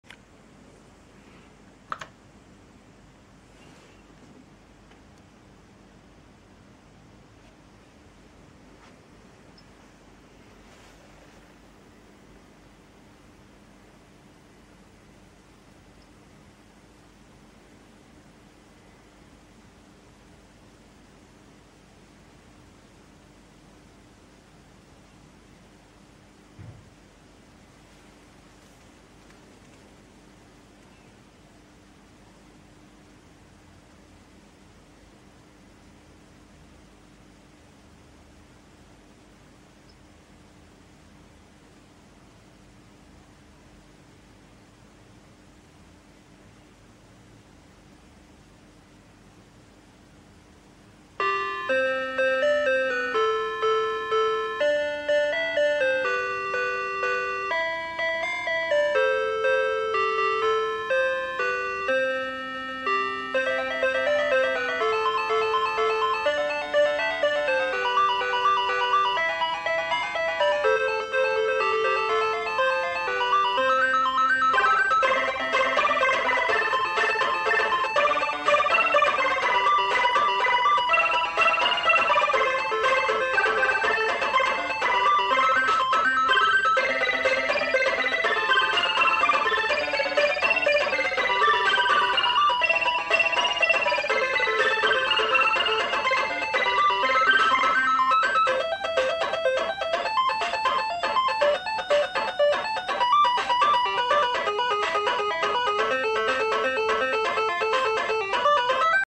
Sarbatori Fericite! 2022
Azilul de Calculatoare vine cu steaua, un program realizat de noi acum in C care ruleaza pe calculatorul Oric Atmos aparut in 1984. Playerul audio si instrumentele utilizate pentru conversie vin cu Oric SDK, muzica este realizata de Fotis SALMAS aka FG Brain din grupul Chaos (Grecia).
This is the real loading time version.